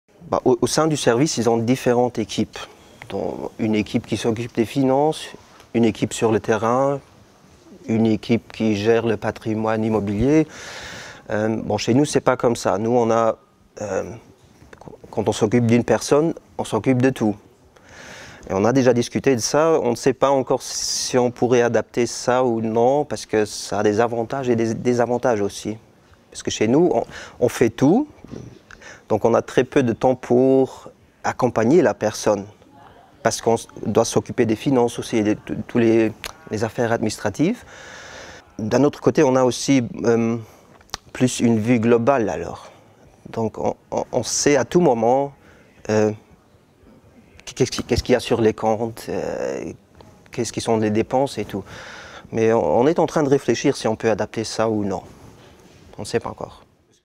Les différences?
Steve FELLENS, du service accompagnement tutélaire au Luxembourg, répond à la question :Les différences?